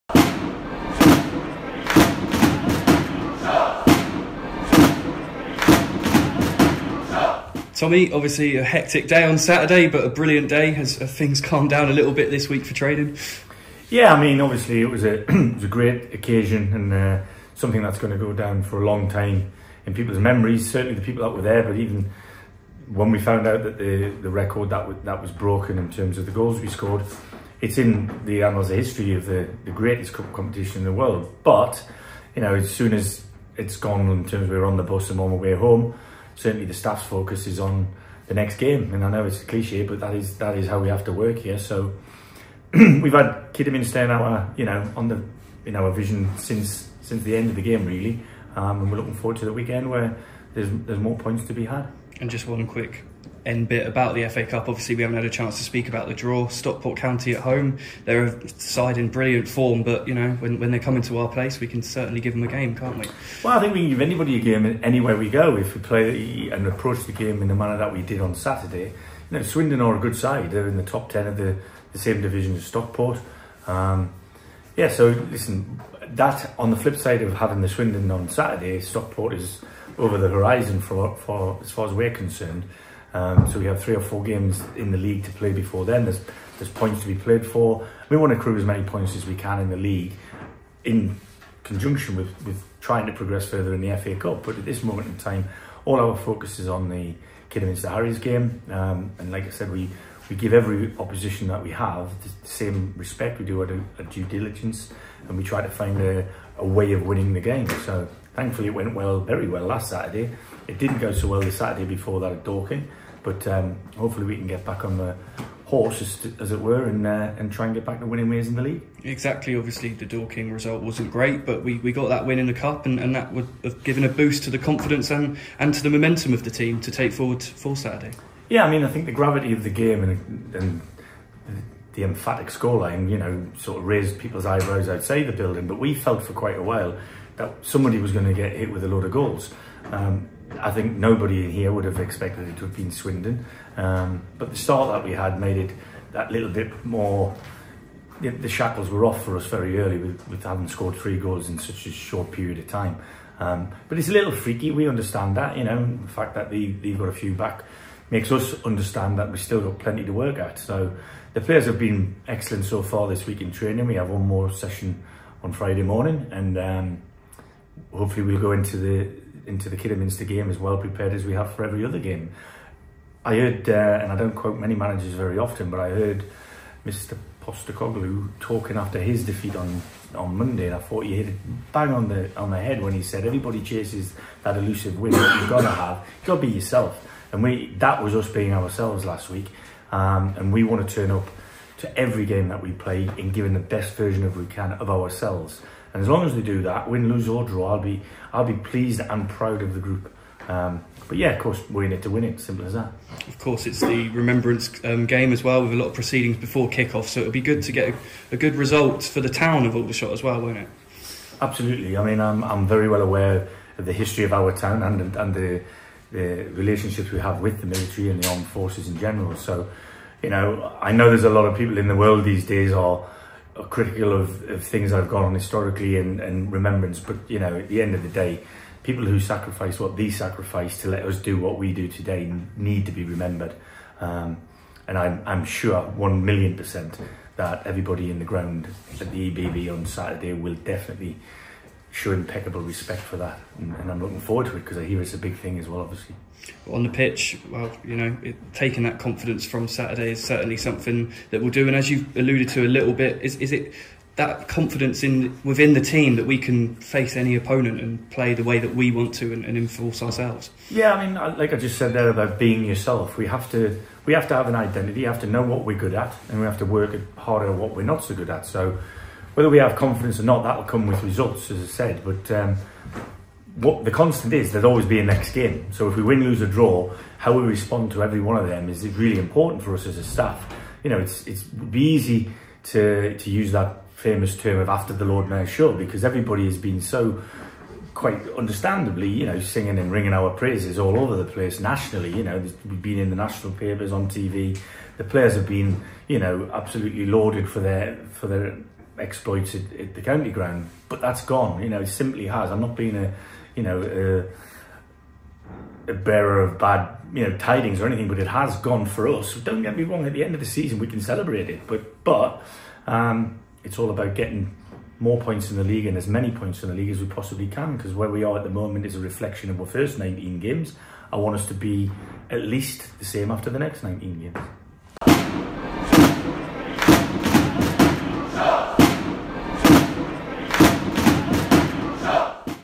TOMMY WIDDRINGTON PRE-MATCH: Kidderminster Harriers (H)
Tommy Widdrington gives his thoughts to ShotsTV ahead of our Vanarama National League fixture with Kidderminster Harriers!